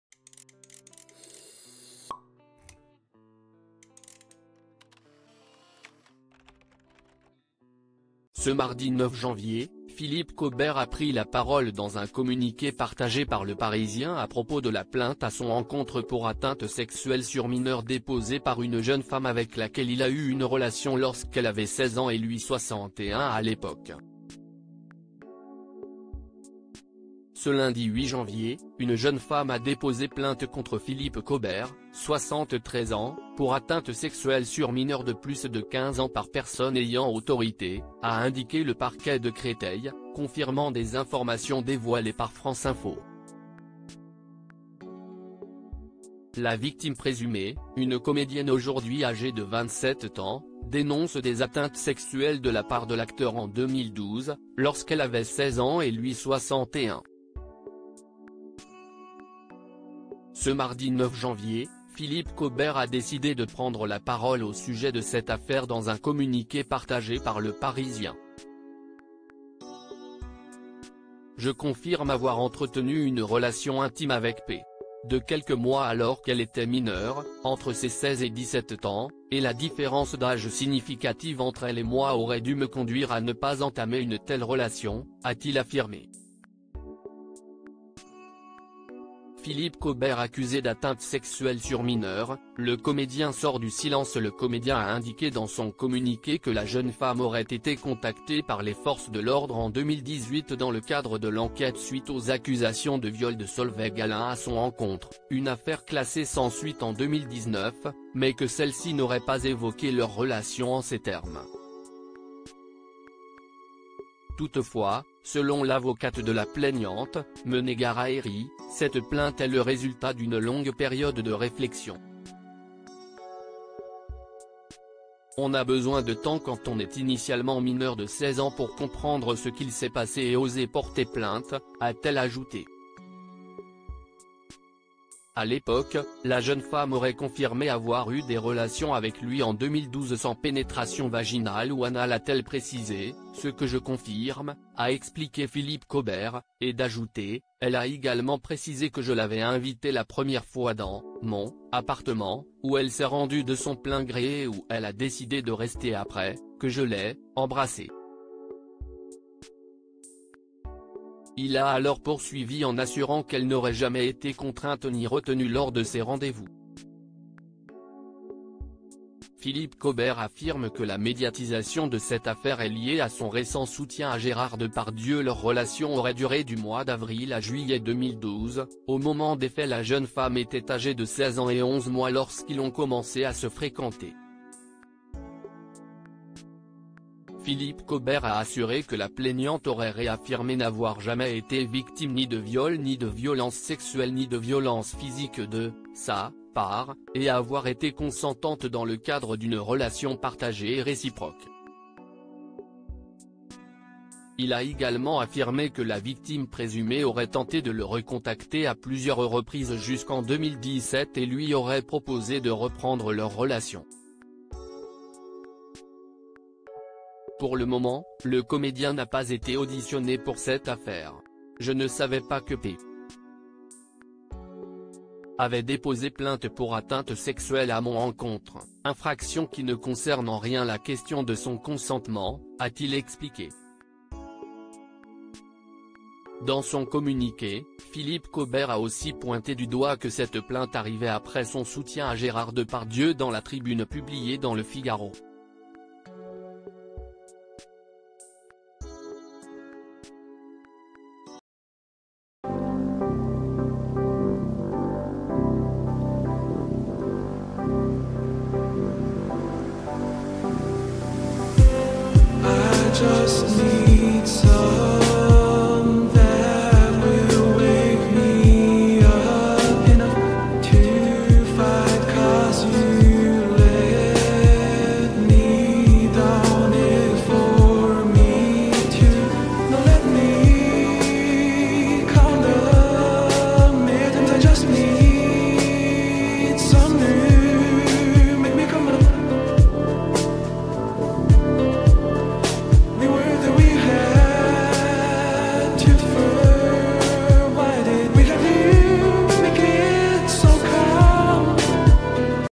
Philippe Caubère accusé d’atteinte sexuelle sur mineure le comédien sort du silence et livre sa
Philippe Caubère accusé "d’atteinte sexuelle sur mineure" : le comédien sort du silence et livre sa version des faits